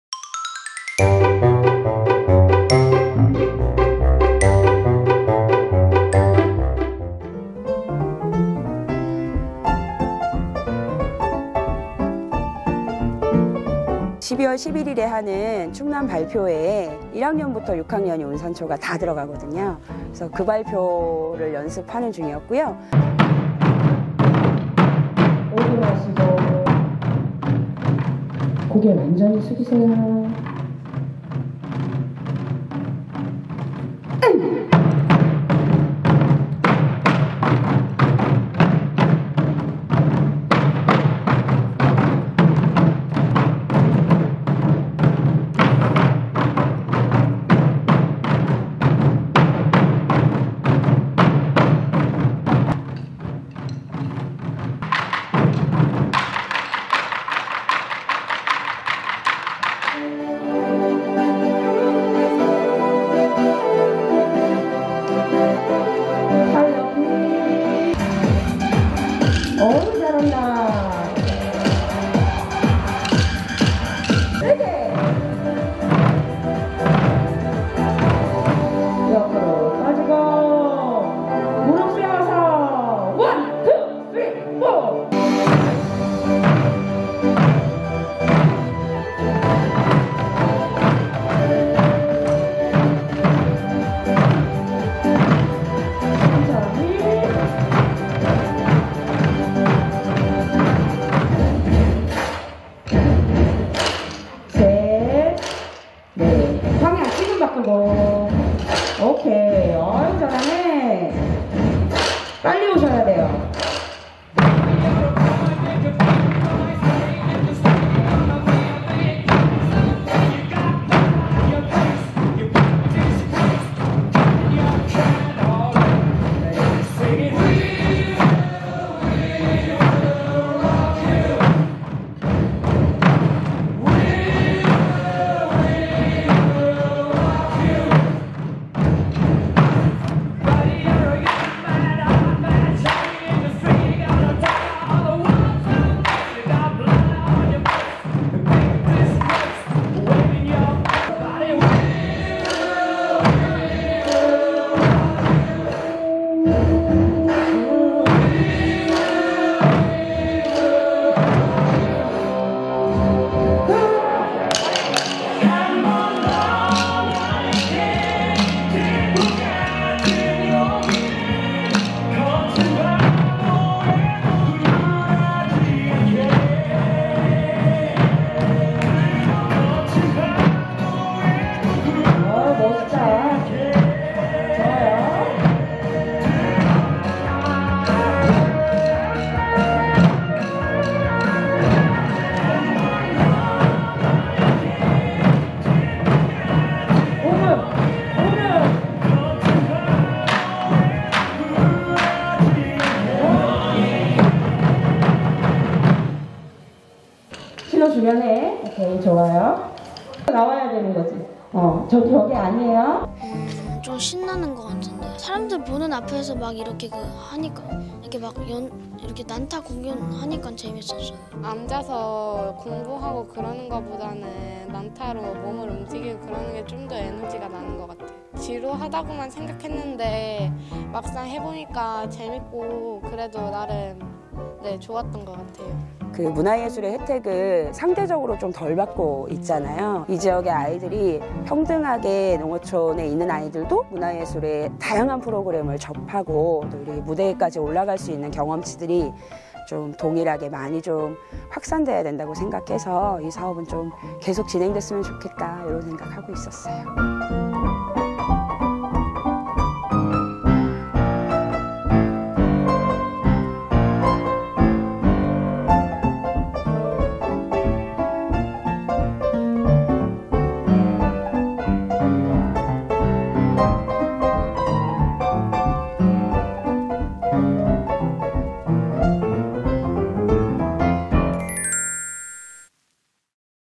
[충남] ep.4 2023 우리동네 예술학교 서산 운산초 난타수업
2023 농어촌학교 문화예술교육 "우리동네 예술학교" 충남 서산 운산초등학교 난타수업 현장입니다